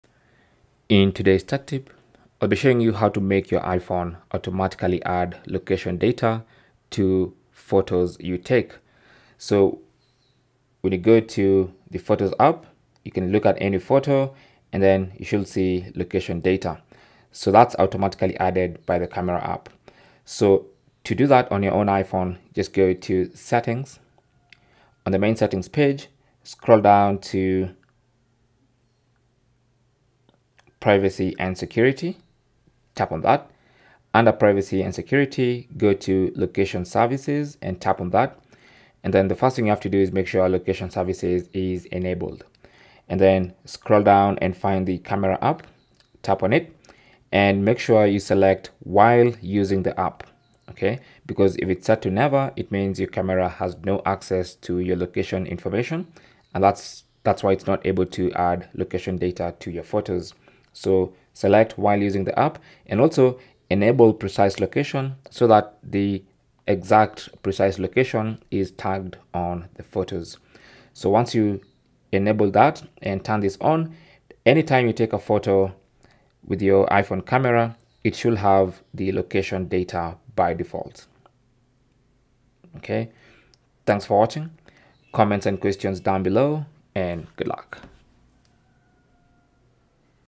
How To Enable Automatic Location Tags On iPhone Photos
Learn how to enable automatic location tags on iPhone photos to add context to your memories and easily remember where each picture was taken.

This feature utilizes your iPhone’s GPS to embed location data into your images, allowing for effortless organization and searching by location. By following a few simple steps, you can ensure that every snapshot captures not just a visual memory, but also the geographical details that make your photo collection even more meaningful.

-Open the main Settings page on your iPhone.
-Scroll down and tap on Privacy & Security.
-Select Location Services under Privacy & Security.
-Turn on Location Services.
-Scroll down to find the Camera app and tap on it.
-Choose While Using the App to allow location access.
-Turn on Precise Location for accurate location tagging.

That is how you  Enable Automatic Location Tags On iPhone Photos.

Cell Phone Tripod Adapter